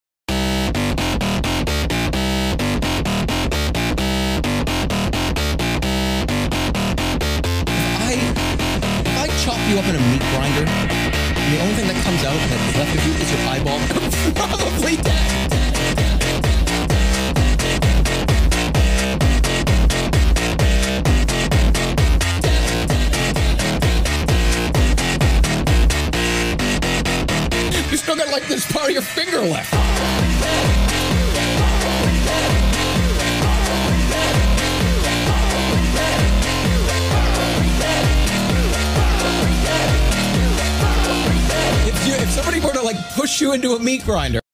Meat grinder meme [Ibispaint animation]
Hiii, I think this is the first animation meme on the channel!

Used:
Capcut
Ibispaint
My fingers

Time taken:
1 day

Character used:
Zonya